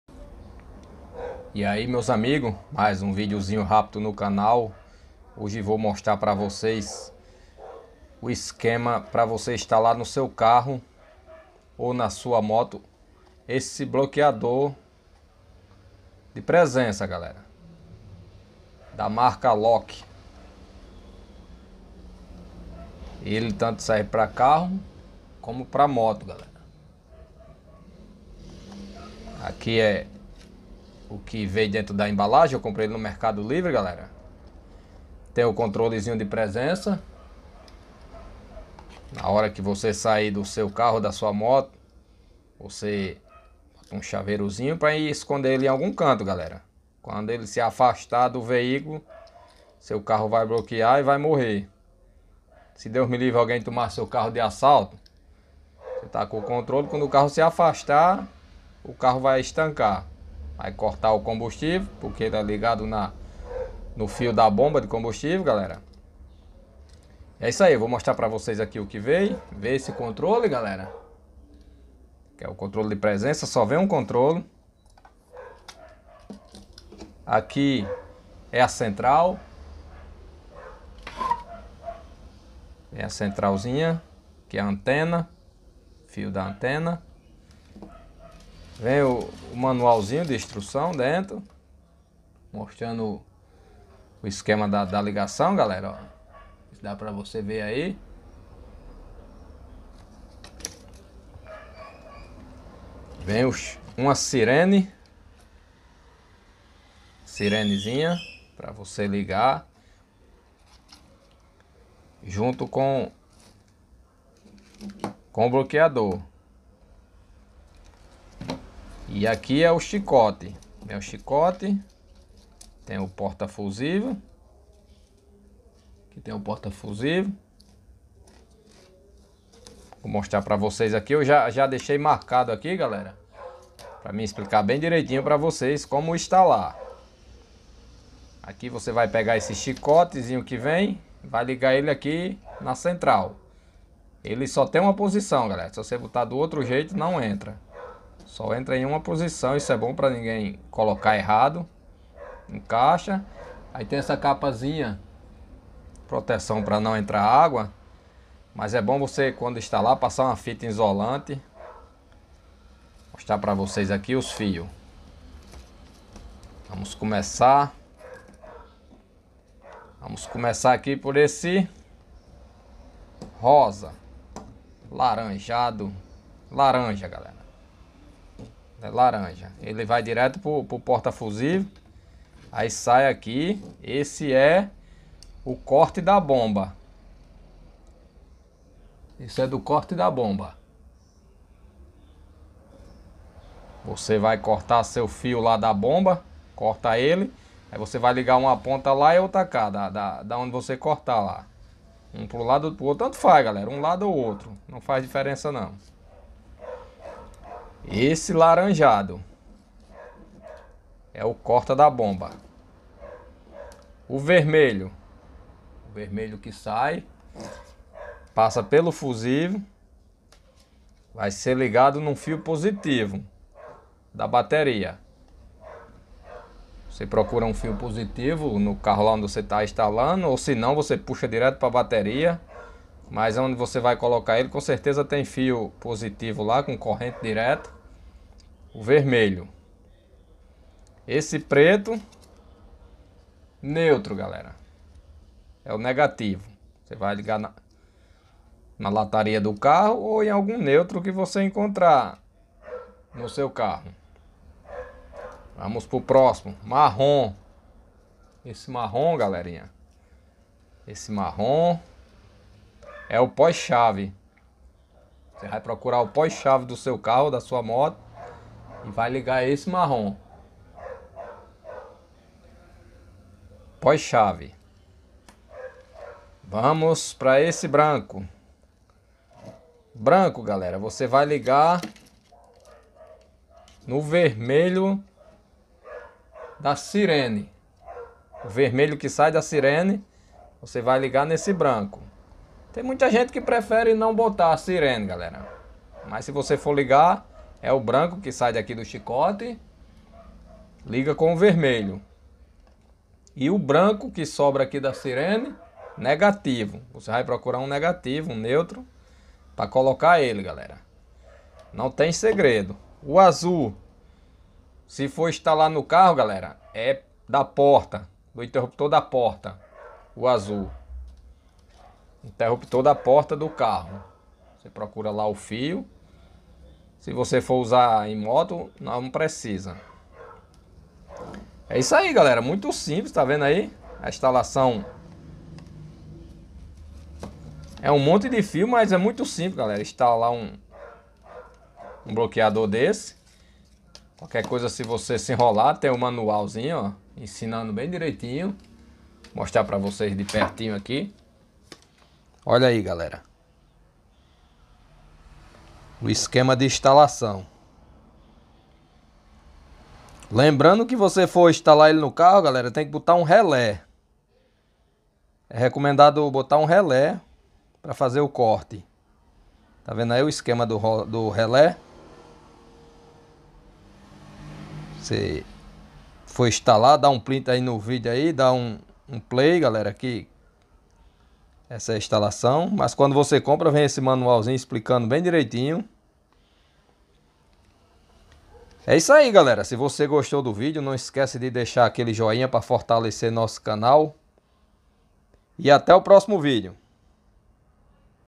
como instalar bloqueado de afastamento de presença
nesse vídeo. vamos mostrar como instalar bloqueado no seu carro e moto